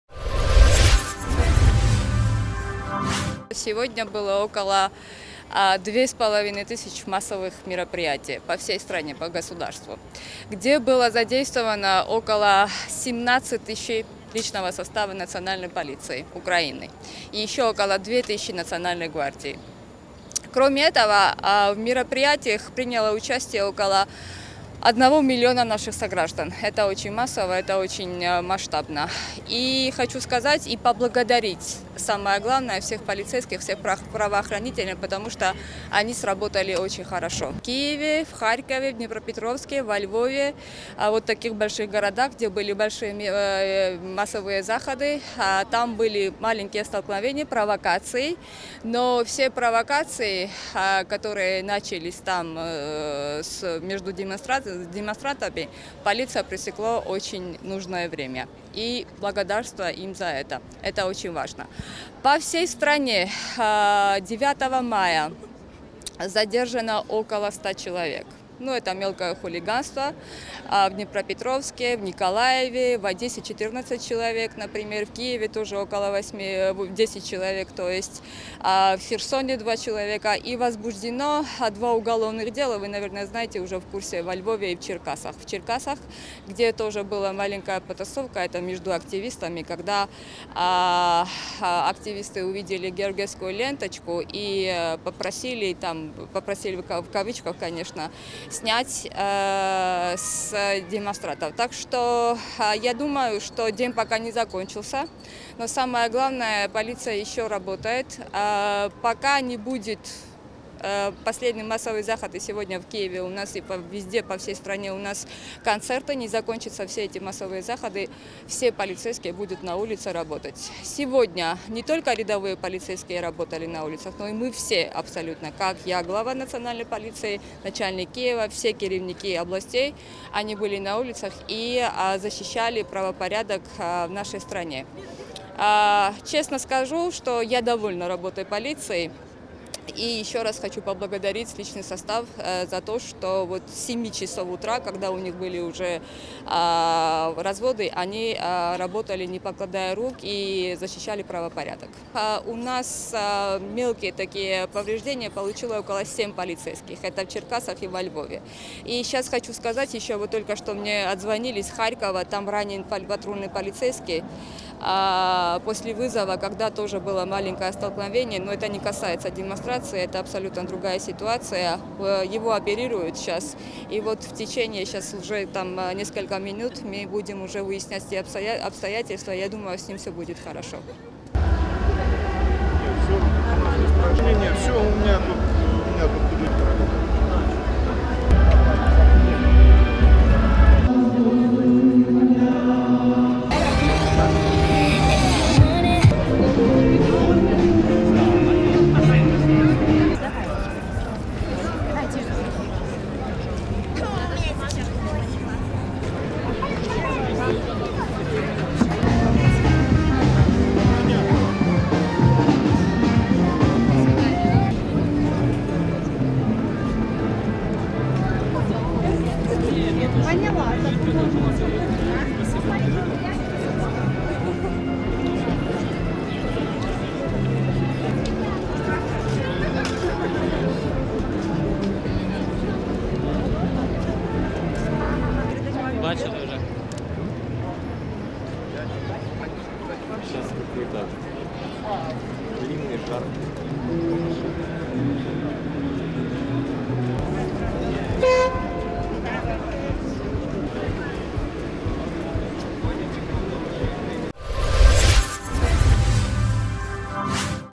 Потужна профілактика з боку поліції - забезпечила спокій громадян 8-9 травня – Хатія Деканоідзе
Поліція належно спрацювала упродовж усіх свят і пам'ятних заходів, що тривають в Україні з 26 квітня до 9 травня. Про це глава Національної поліції України Хатія Деканоідзе повідомила сьогодні, 9 травня, під час брифінгу на Майдані Незалежності в Києві.